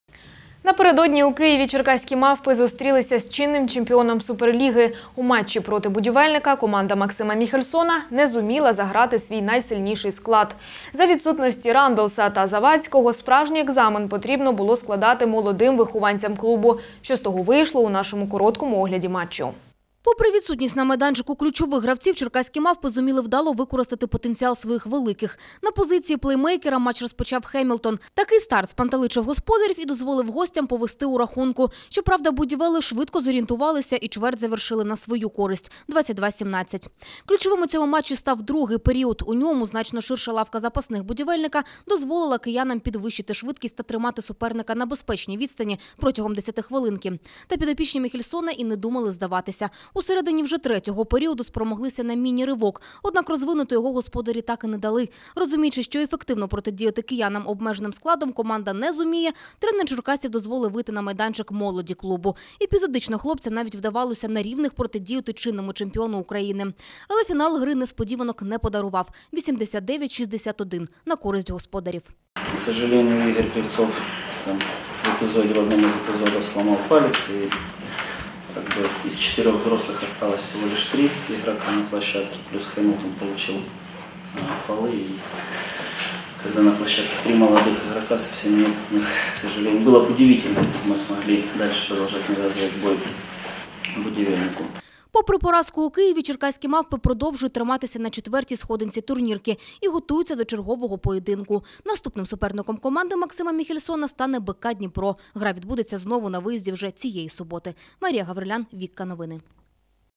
«Черкаські Мавпи» поступилися чинному чемпіону Суперліги
Напередоні у Києві «Черкаські Мавпи» зустрілися з чинним чемпіоном Суперліги. У матчі проти «Будівельника» команда Максима Міхельсона не зуміла заграти свій найсильніший склад. За відсутності Рандлса та Завадського справжній екзамен потрібно було складати молодим вихованцям клубу. Що з того вийшло – у нашому короткому огляді матчу.